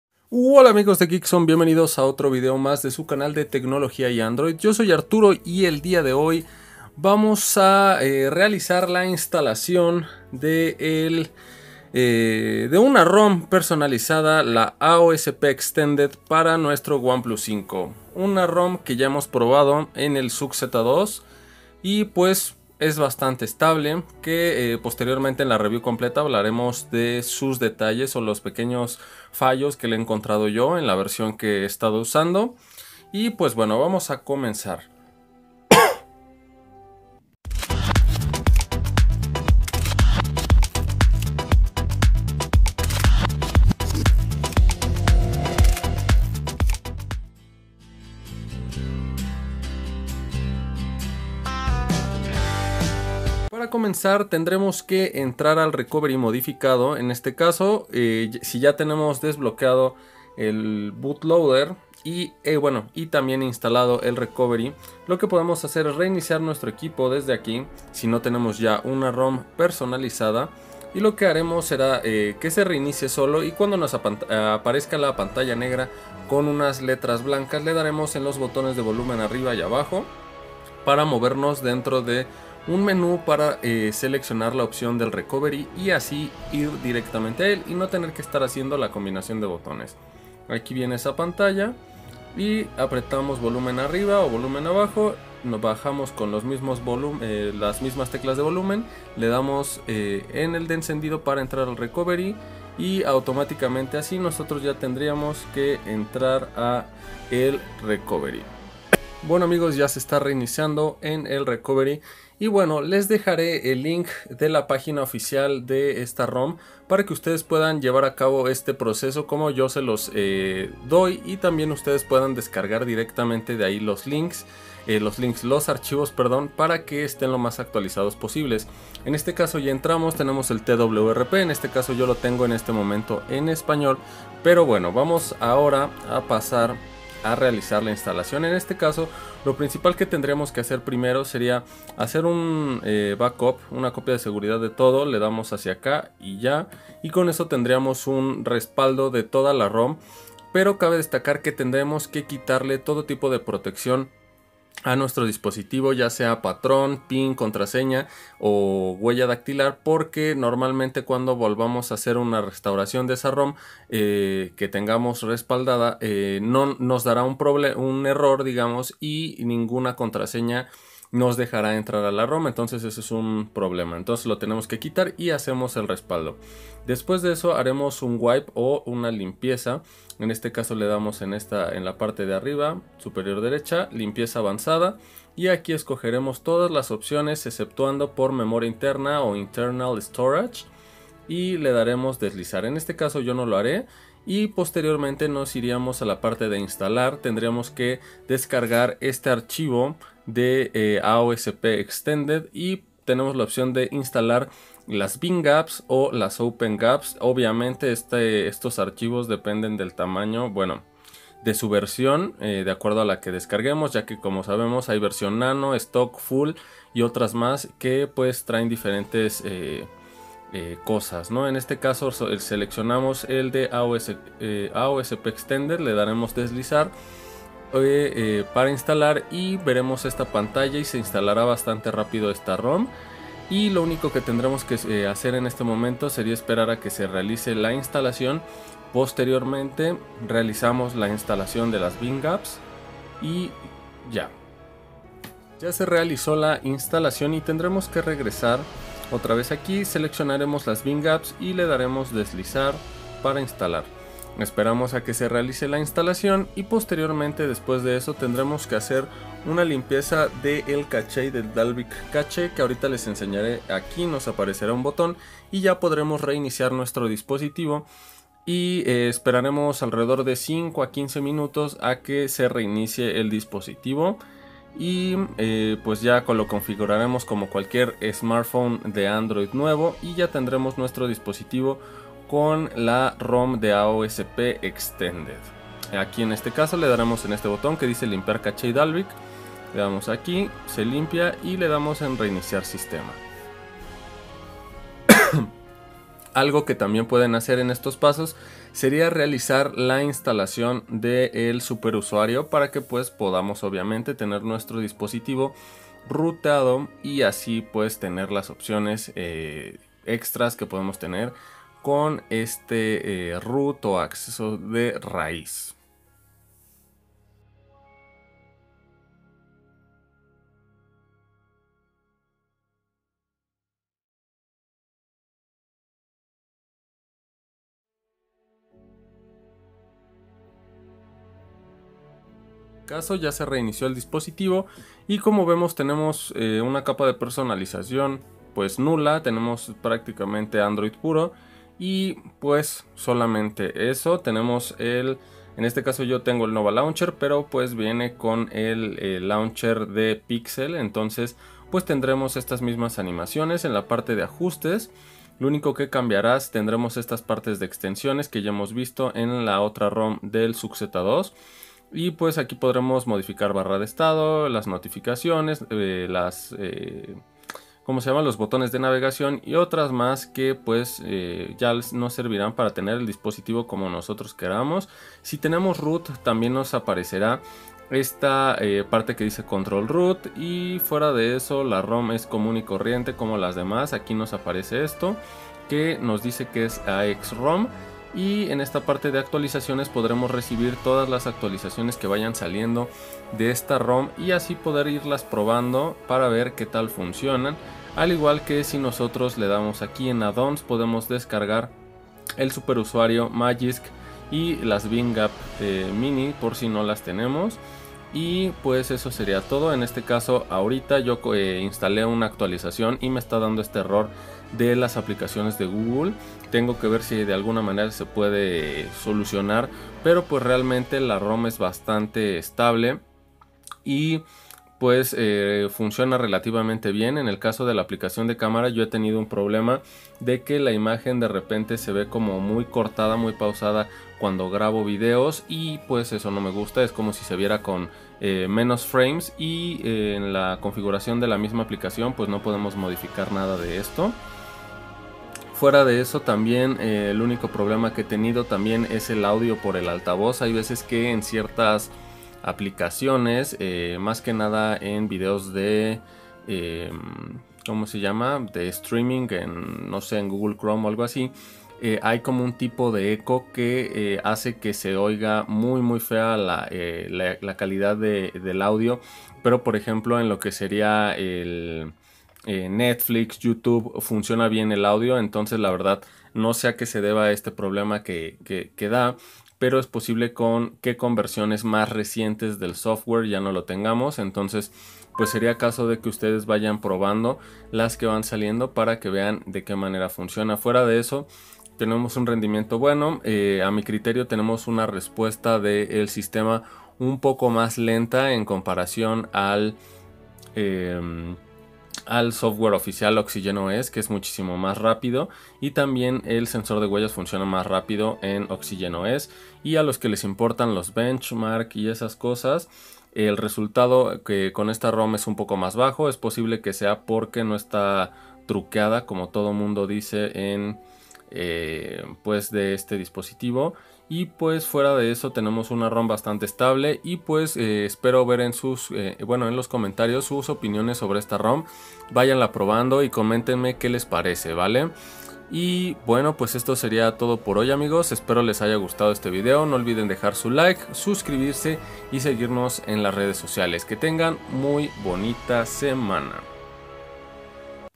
AOSP Extended Rom Estable / Oneplus 5
En este nuevo video, amigos les enseñaré como instalar la rom AOSP Extended para el Oneplus 5.

Una gran pm con buen rendimiento y estabilidad con la cual tendrás una experiencia bastante buena y diferente a la rom oficial de Oxygen OS.

No te lo puedes perder, cualquier duda dejarla en los comentarios o consultar al correo electrónico del canal.